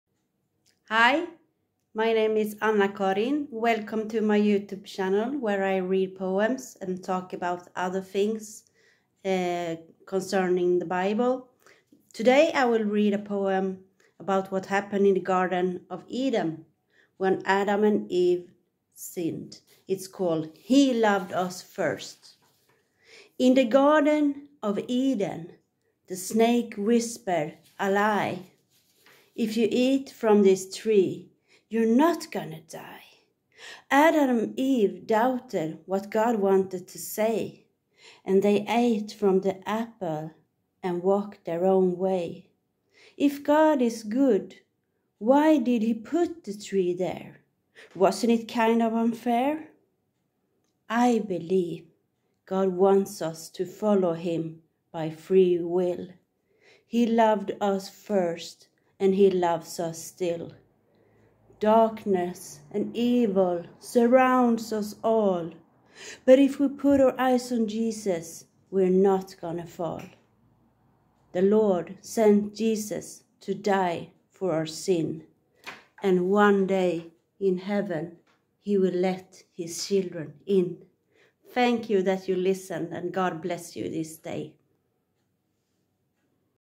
Poem- He loved us first 💖
He loved us first

In the garden of Eden the snake whispered a lie
If youeat from this tree you're not gonna die!
Adam and Ewe doubted what God wanted to say
And they ate from the Apple and walked their own way
If God is good, why did he put The tree there?
Wasnt it kind of unfair?
I believe God wants us to follow him by free Will
He loved us first and he loves us still
Darkness and evilsurrounds us all
But If we put our eyeson Jesus we are not gonna fall!
The Lord sent Jesus to die for our sin
And one Day in heaven he will let his children in!

Anna-Karin Stenberg